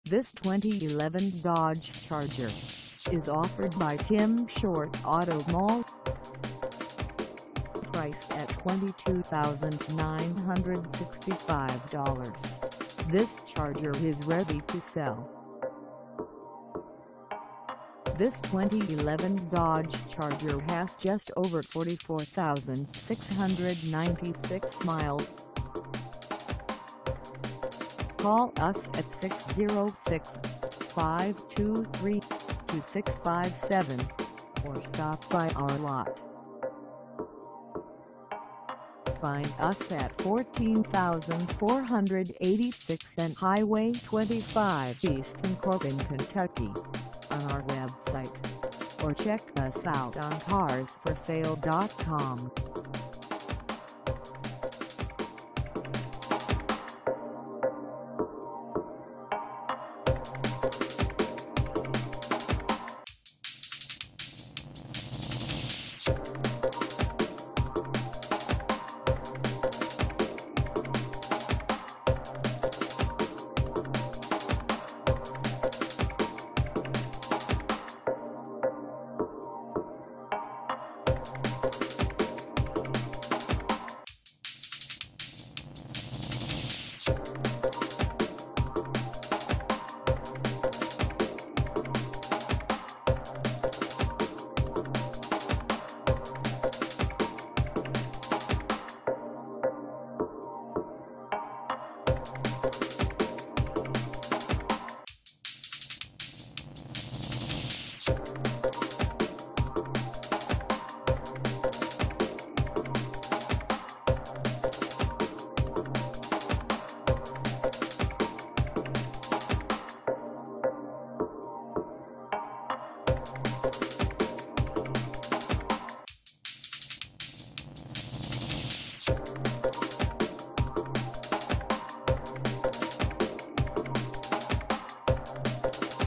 2011 Dodge Charger R/T 4dr Sedan for sale in Corbin, KY 4070
This 2011 Dodge Charger R/T 4dr Sedan is for sale in Corbin, KY 40701 at Tim Short Auto Mall .
Engine: V8 5.7L V8
Transmission: Automatic 5-Speed
Color: Bright White Clearcoat
Mileage: 44696
Contact Tim Short Auto Mall  14486 N. Hwy. 25 E Corbin, KY 40701 to test drive this 2011 Dodge Charger R/T 4dr Sedan today.